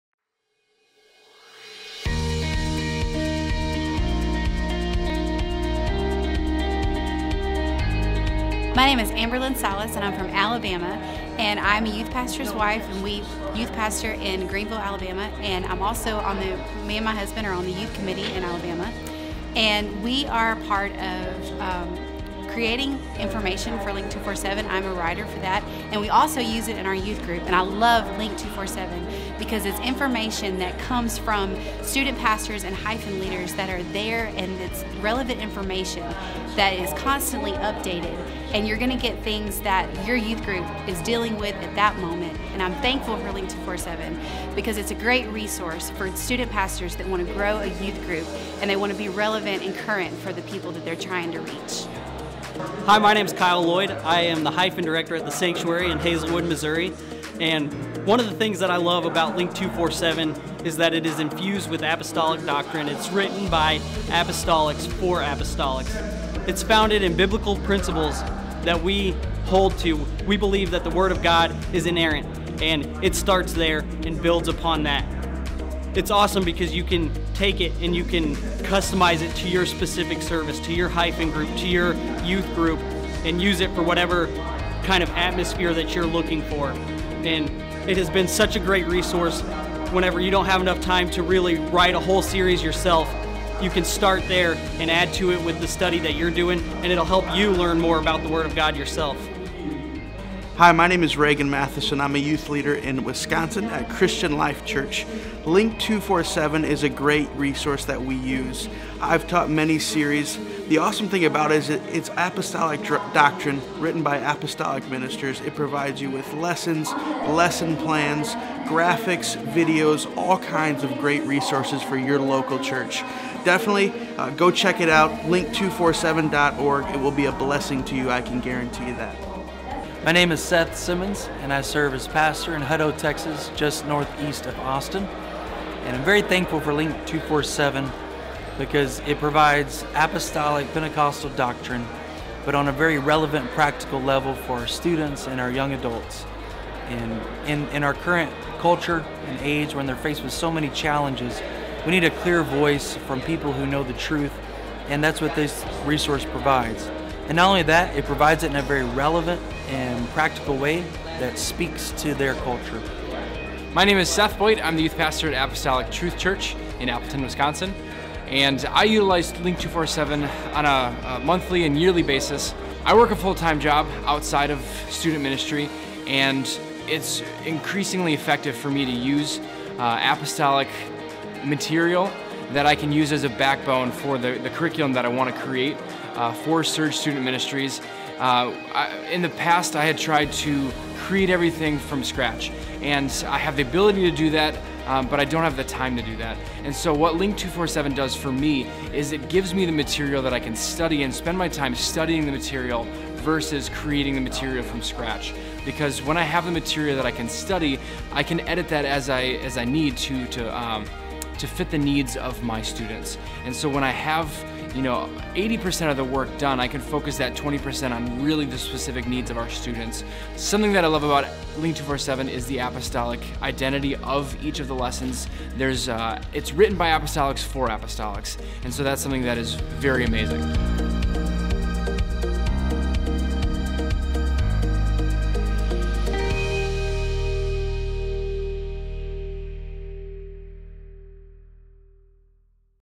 LINK247 2016 Promo
Check out what hyphen and youth leaders are saying about the all-digital curriculum LINK247.